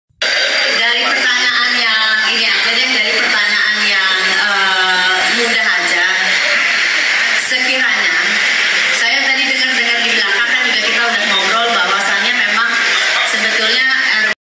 Wanda Hamidah
Wanda Hamidah nan cantik rupawan ini ialah model, aktris, aktivis, dan politisi dengan senyuman yang manis dan tatap matanya yang sangat menawan.
Sukai laman kami di dan simaklah beragam video Wanda Hamidah.